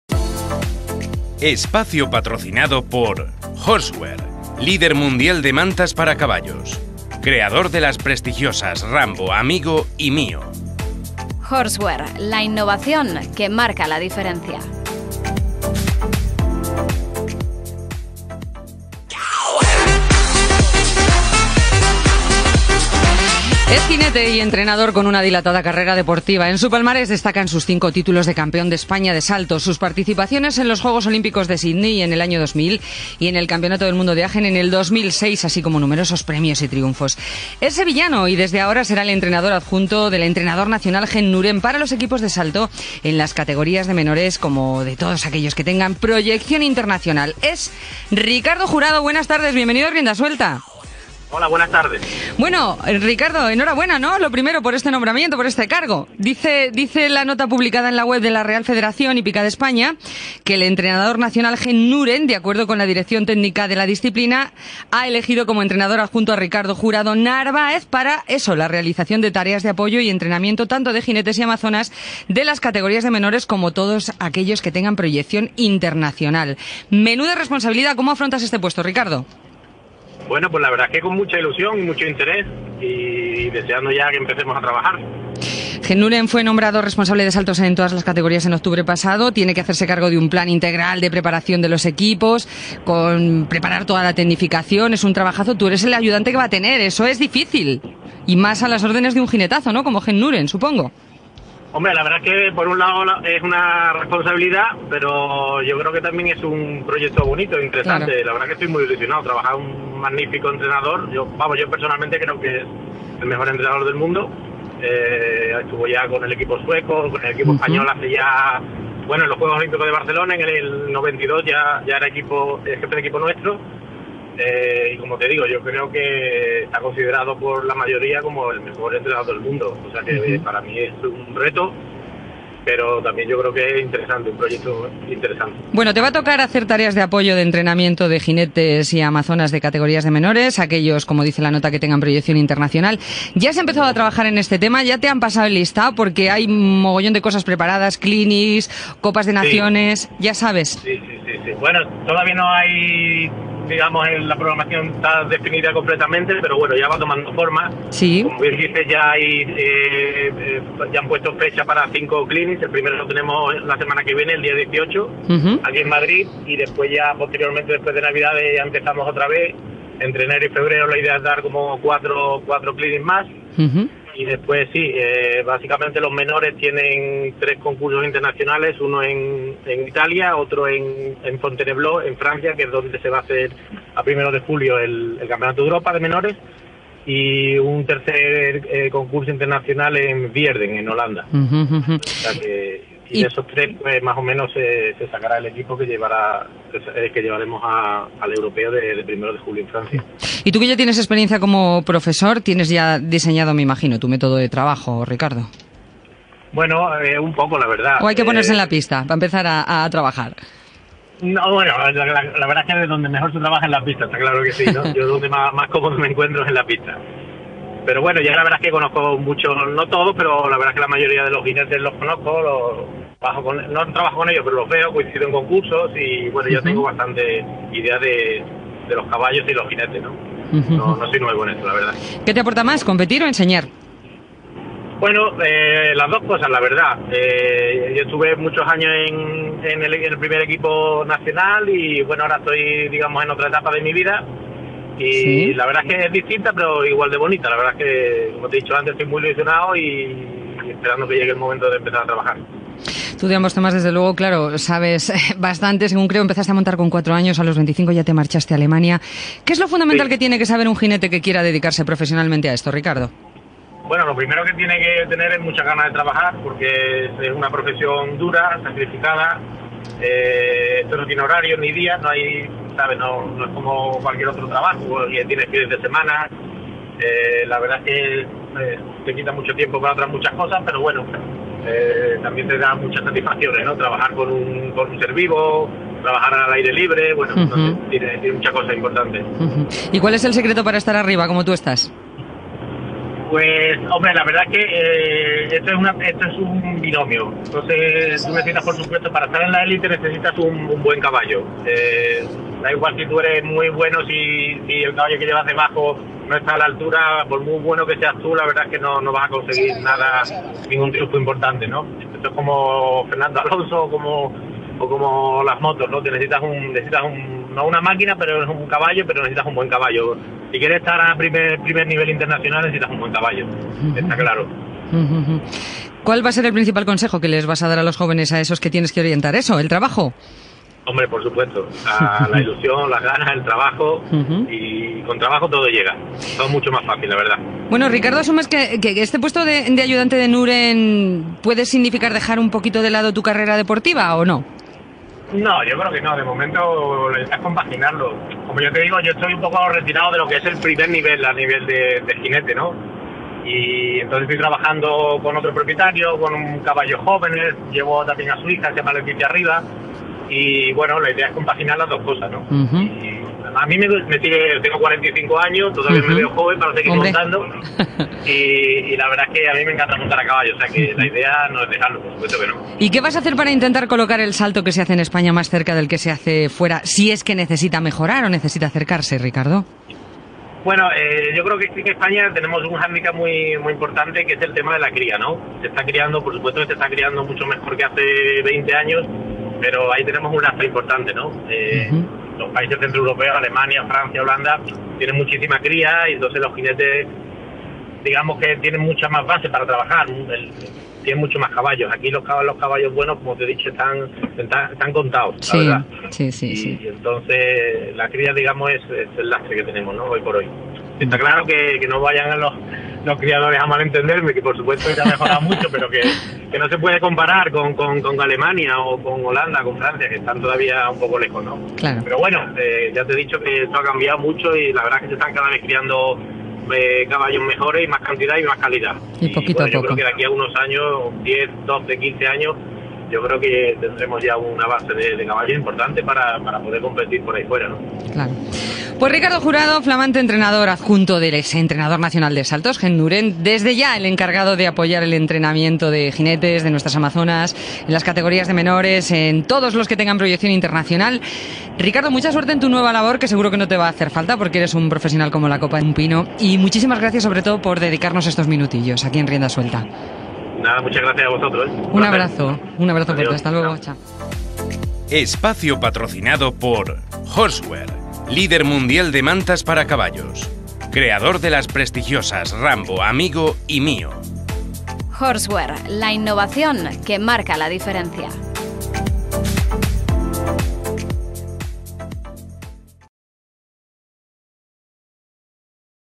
Ricardo Jurado: “Es un proyecto interesante”
Ricardo Jurado será la mano derecha del entrenador nacional de Saltos de la Real Federación Hípica Española, el holandés Henk Nooren. Jurado habla de su nuevo reto en Rienda Suelta Radio.